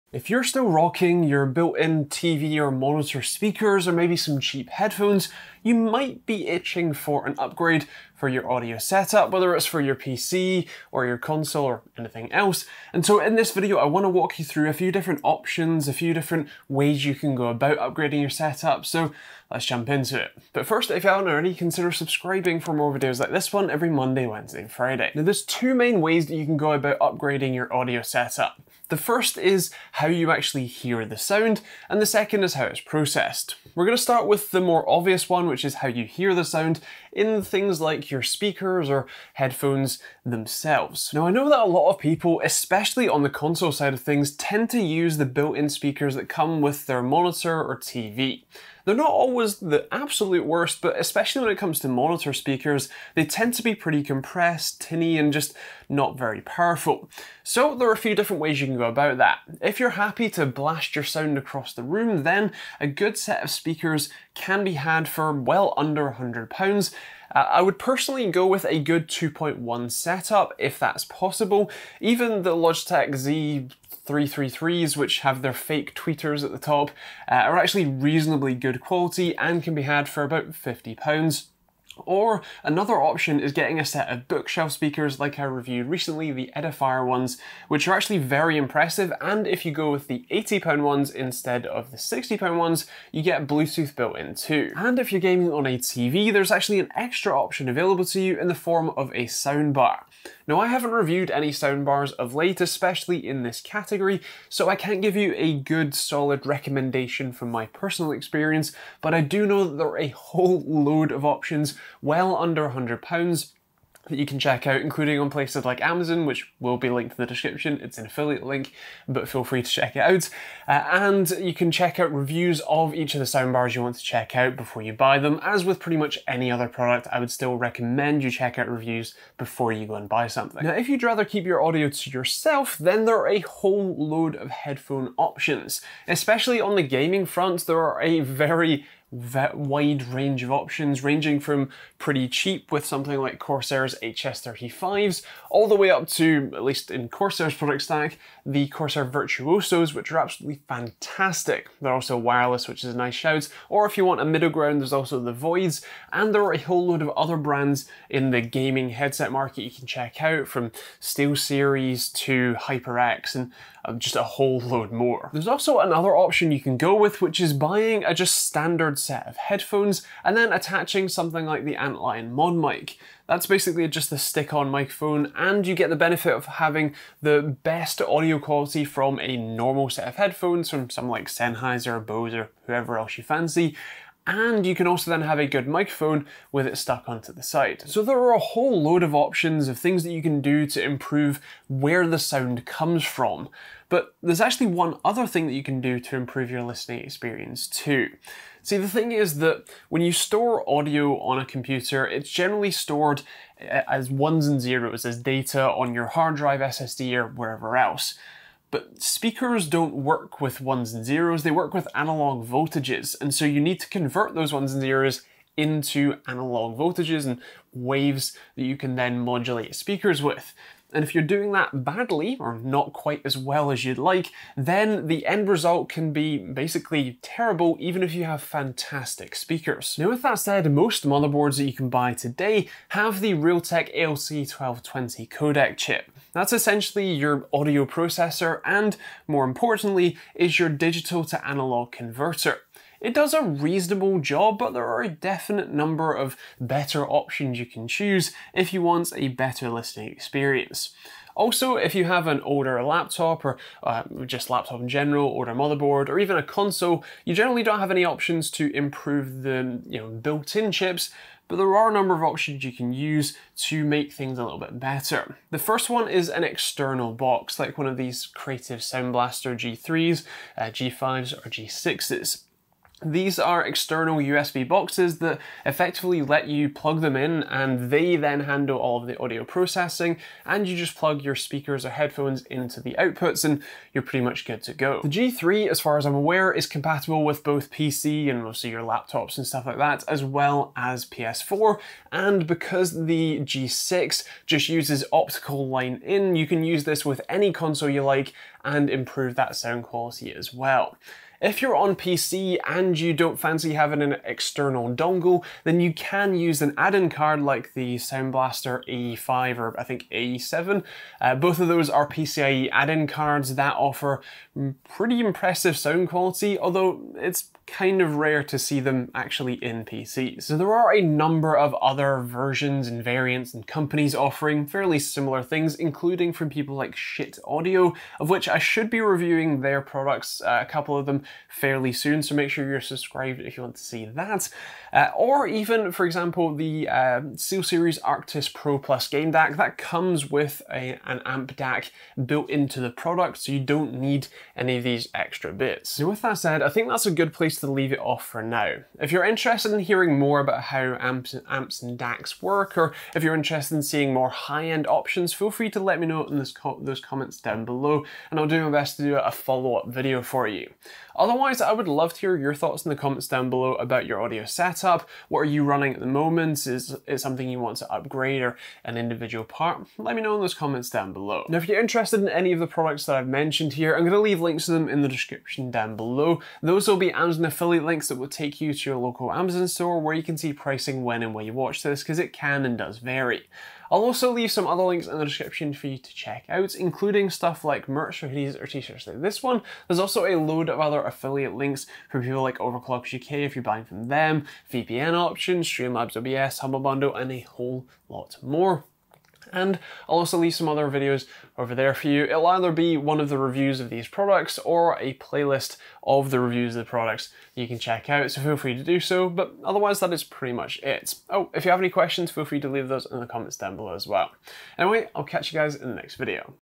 How to Upgrade your PC or Console’s Audio - TechteamGB
If you are still rocking your TV or monitors’ built in speakers, cheap headphones or hand-me-down speakers, you might want to consider an upgrade to your audio setup. In this video, I want to walk you through the different ways you can do that and give you some options to consider, so let's jump into it.